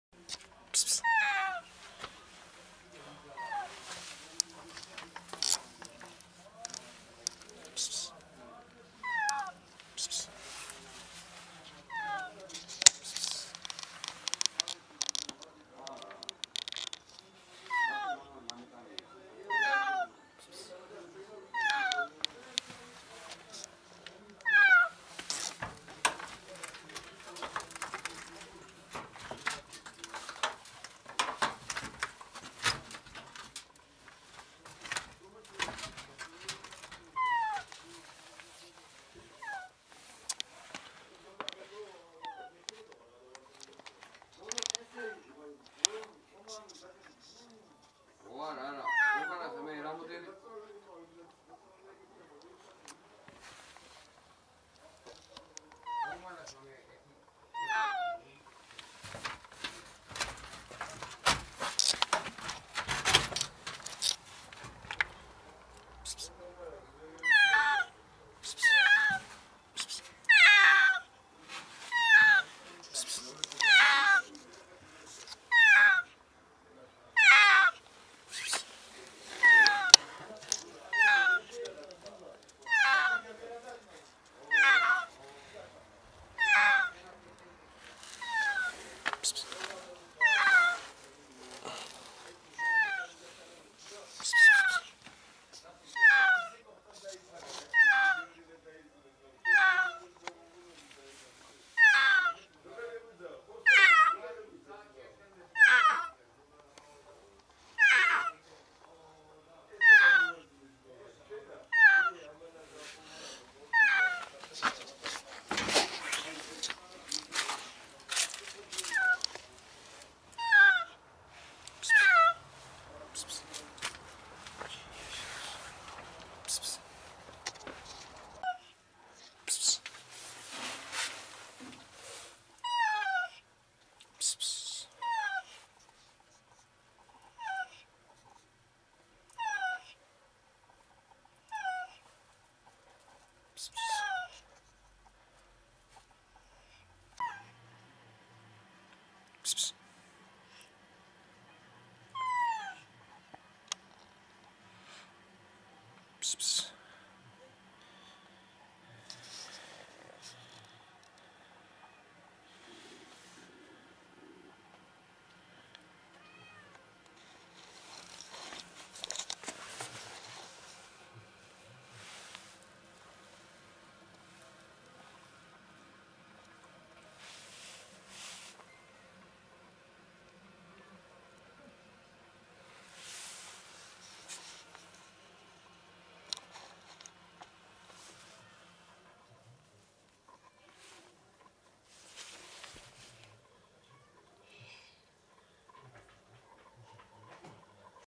Cat trapped in an alley desperately meows for help
I was just minding my own business on a balcony when I saw (heard desperate meows) this cat trapped on the ground floor in an alley that is accessed only through a door. The cat could not have gotten there unless she was trying to jump over a wall to the other side and fell in there. I opened the door for the cat and she seems extremely sick. I gave her food and water but she could not eat. She could not even walk properly. Clearly needs vet attention. Unfortunately, I do not have financial aid to do so, it all depends on people donating. In my country, there is a curfew after 9pm and I certainly cannot take her to a vet or anything at night. I will see what I can do tomorrow.